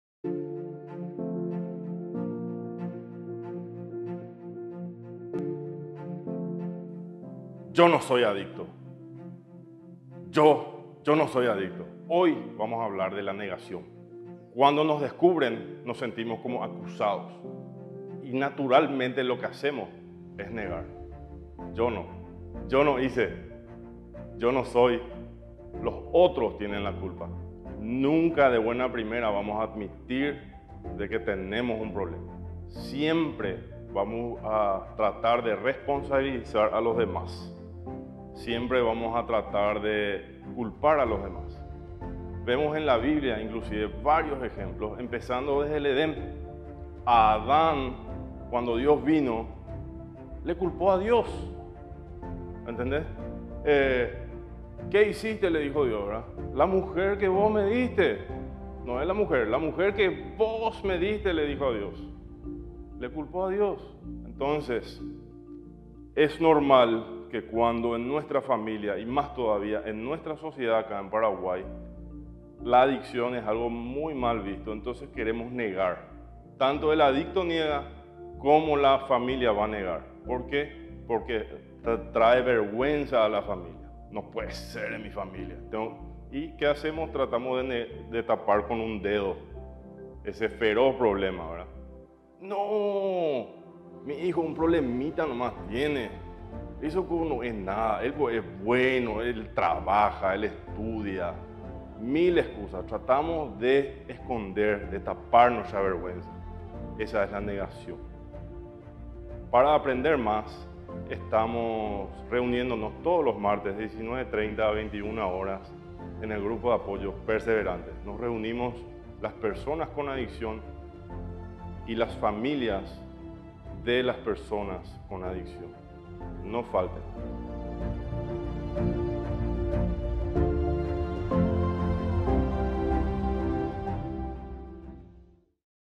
PER012 Serie Etapas de la adicción | 1. La Negación
Cuando nosotros, o alguien cercano a nosotros, cae en adicción, la primera reacción tiende a ser la negación, que es una manera de minimizar la situación. El problema es que cuando hacemos esto, en realidad nos estamos alejando más de la salida. ¿Estás negando una situación de adicción? Mira este video.